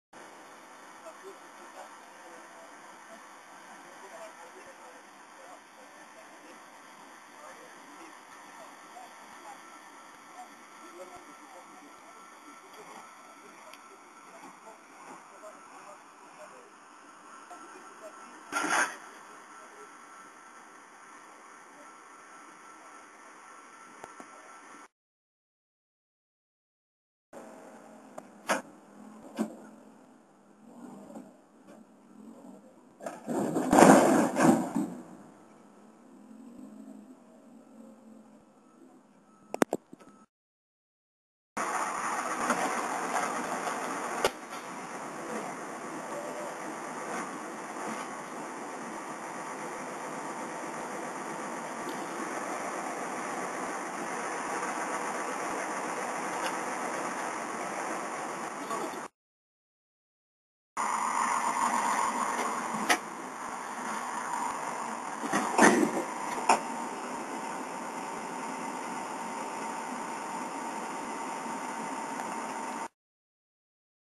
Camiao_Final.wmv
Camião tombou na estrada entre Elvas e Campo Maior.